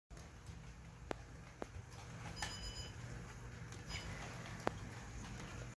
Slo mo video of SOU E8A at 28% track faults 2
Slow motion video of SOU E8A entering Snap-Switch. First cab shifts up as it rolls over points and Main Closure rail. Then outside front wheel bounces on rail.  Digitrax Booster Track Fault can be heard faintly.